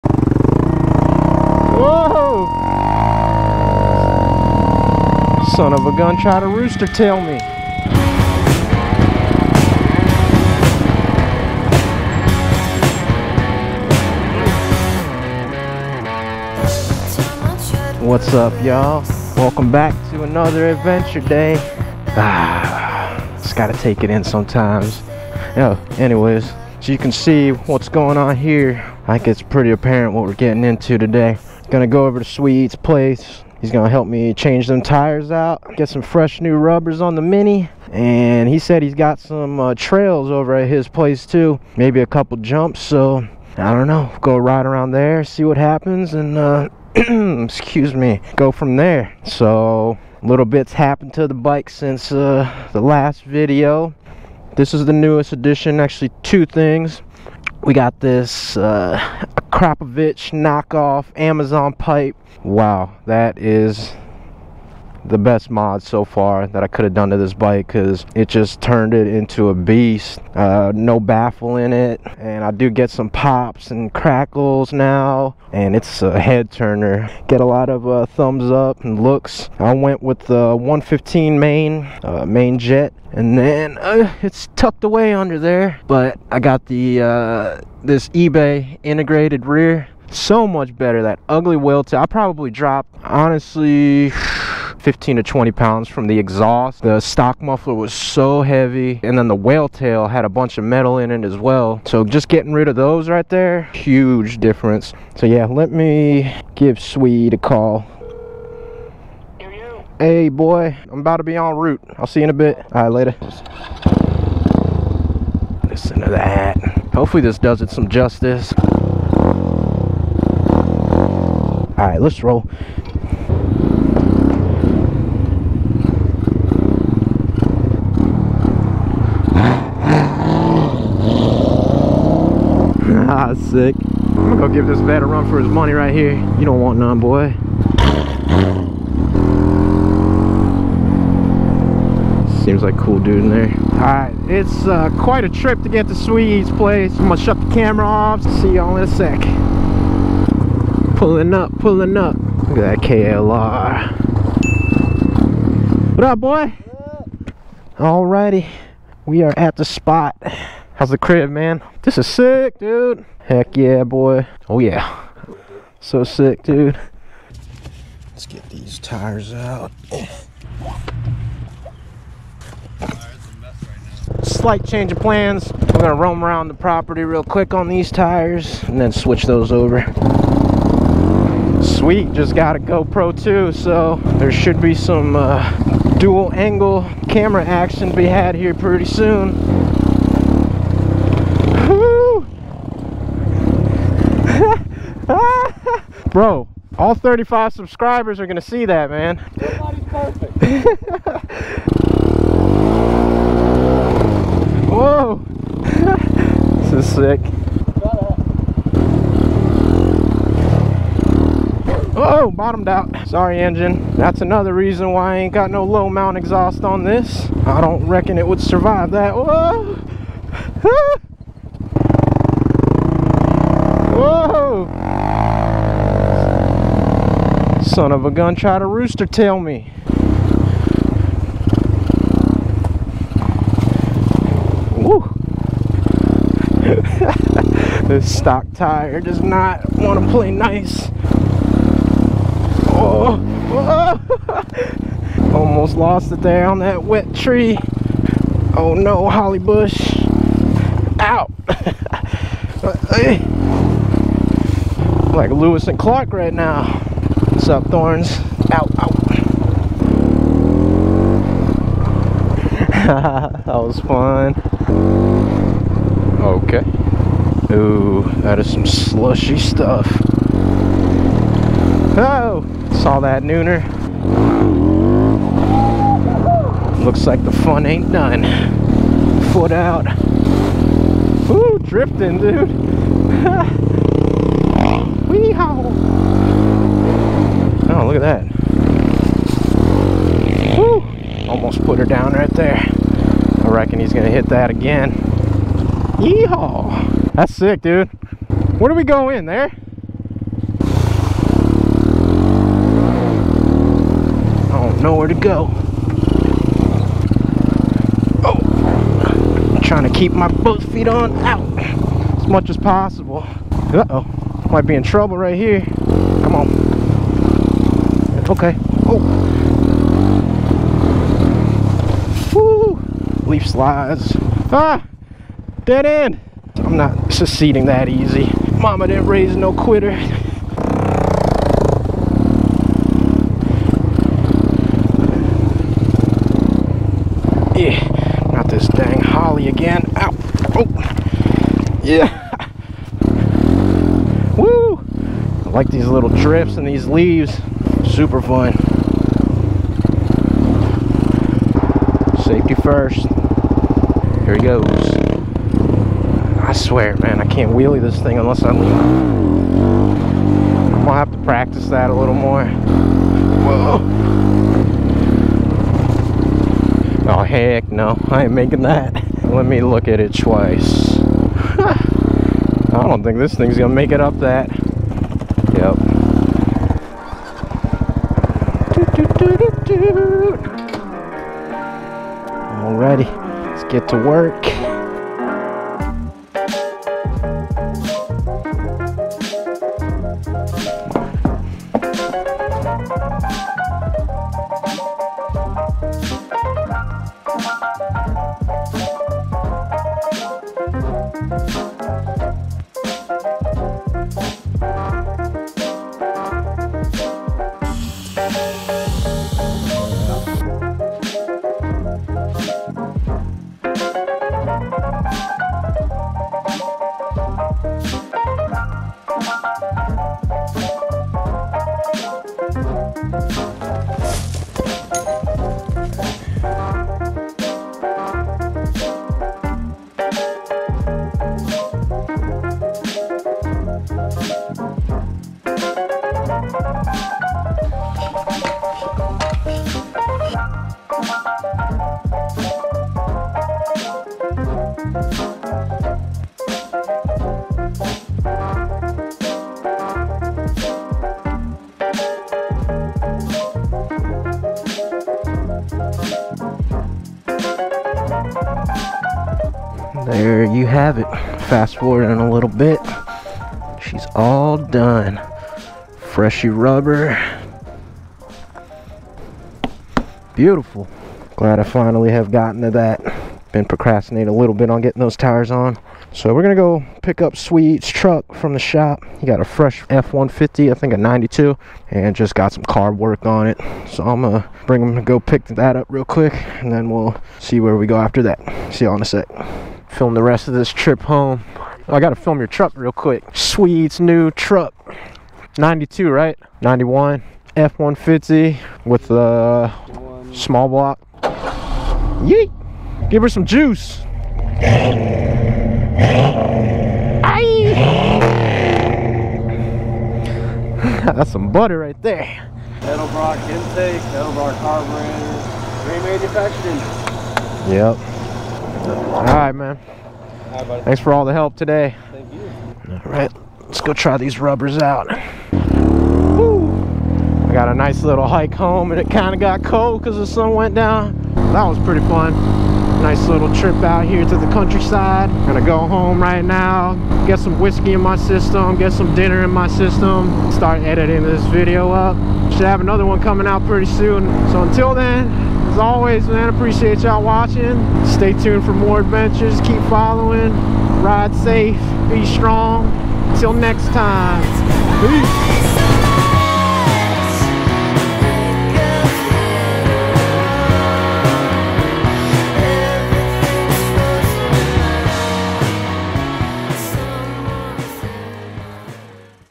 Masked Avenger Ep. 14: Changes
1440p for best results

a trip to the countryside on the mini to give it some new shoes :D